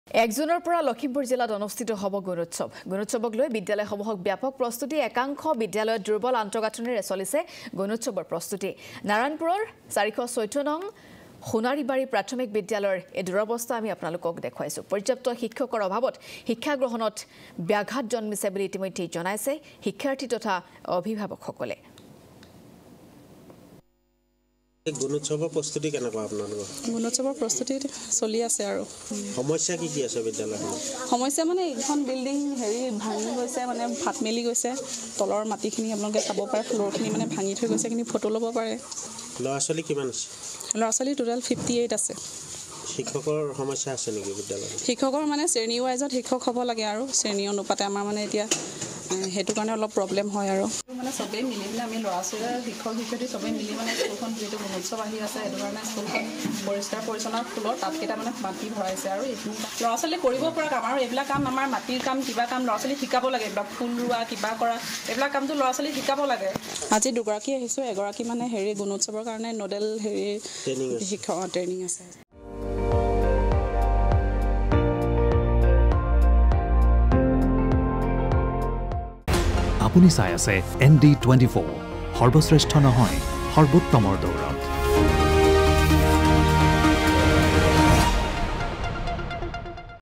নাৰায়ণপুৰৰ ৪১৪ নং সোণাৰিবাৰী প্ৰাথমিক বিদ্যালয়ৰ দুৰৱস্থা
নাৰায়ণপুৰৰ ৪১৪ নং সোণাৰিবাৰী প্ৰাথমিক বিদ্যালয়ৰ দুৰৱস্থা। পৰ্যাপ্ত শিক্ষকৰ অভাৱত শিক্ষা গ্ৰহণত বাধা শিক্ষাৰ্থীৰ।